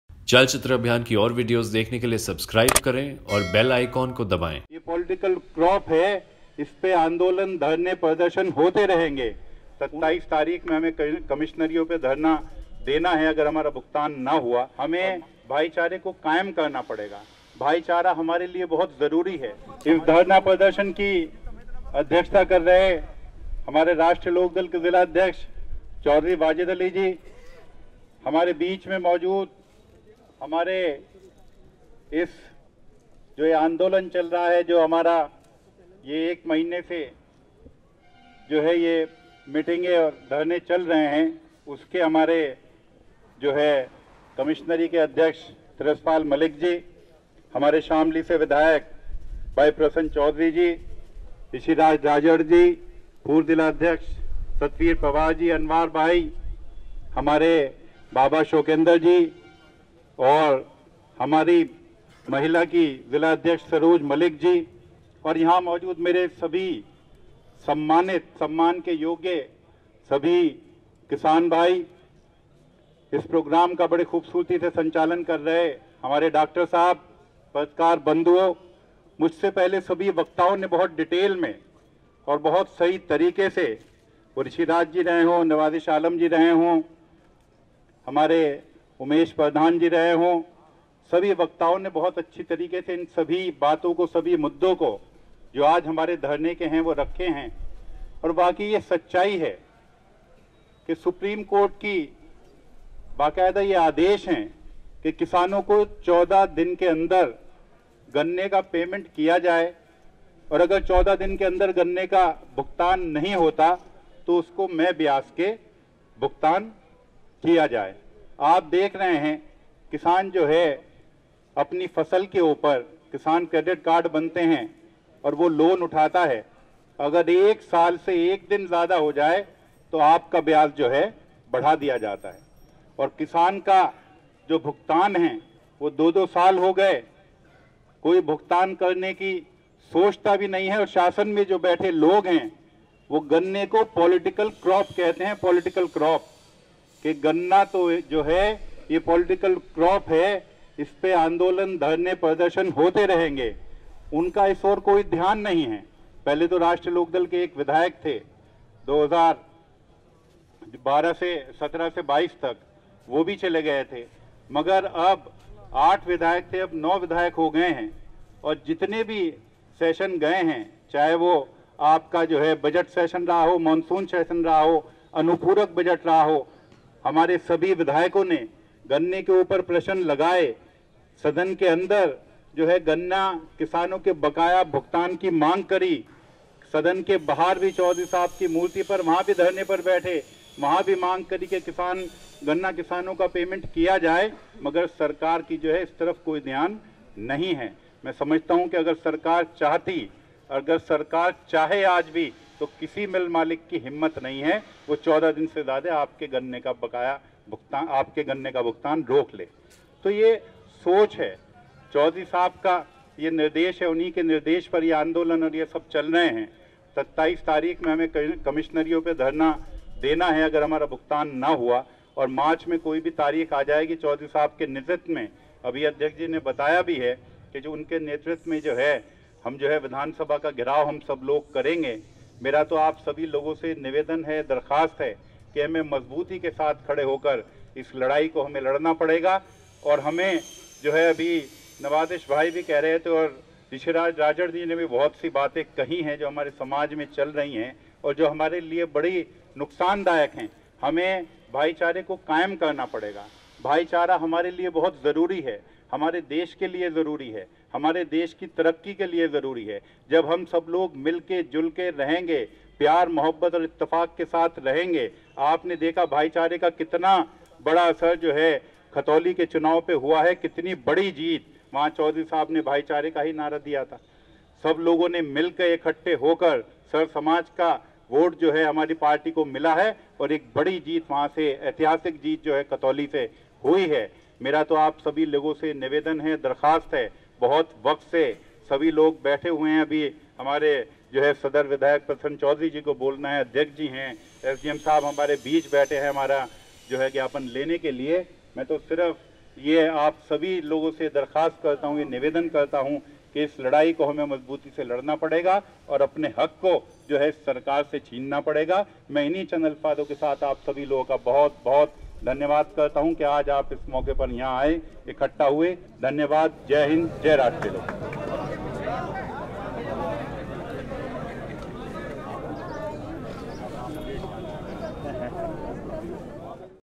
शामली में किसानों के हक में दहाड़े अशरफ अली || Ashraf Ali roars for farmers in Shamli - A CCA Report
राष्ट्रीय लोकदल ने उत्तर प्रदेश में ज़िला शामली   के कलेक्ट्रेट पर गन्ना मूल्य घोषित करने, बकाया गन्ने का भुगतान और निराश्रित पशुओं की समस्या की मांग को लेकर 07 फरवरी 2023 को रालोद विधायकों ने पार्टी कार्यकर्ताओं के साथ कलक्ट्रेट पर प्रदर्शन किया। इस धरने को थानाभवन विधायक अशरफ अली ने संबोधित किया। अशरफ अली ने कहा हैं कि वर्तमान समय में किसानों की हालत बेहद खराब है। न तो उसे गन्ना मूल्य का पता है और न ही पूर्व के वर्षों का बकाया गन्ना भुगतान हुआ है। निराश्रित पशु भी किसानों की फसलों को नष्ट कर रहे हैं। यह अन्नदाता के साथ अन्याय है। उन्होंने कहा हैं कि किसानों की मांगों को पूरा नहीं किया गया तो रालोद एक बड़ा आंदोलन करेगा।

CCA टीम की रिपोर्ट।
टीम - मौ. सावेज़ चौहान, विशाल स्टोनवॉल, इकरा अय्यूब सैफी

On 07 February 2023, RLD MLAs along with party workers demonstrated at the collectorate, demanding the declaration of sugarcane rates, payment of arrears and the problem of destitute animals in Shamli, Uttar Pradesh. Thana Bhavan MLA Ashraf Ali addressed this dharna. Ashraf Ali said that at present the condition of the farmers is very bad. They are neither aware of the sugarcane rates nor has the cane dues of the previous years been paid. Destitute animals have been also destroying the crops of the farmers. This is injustice to the food giver. He said that if the demands of the farmers are not met, RLD will launch a big movement.

CCA team report.
Crew - Mohd Savez Chauhan, Vishal Stonewall, Iqra Ayyub Saifi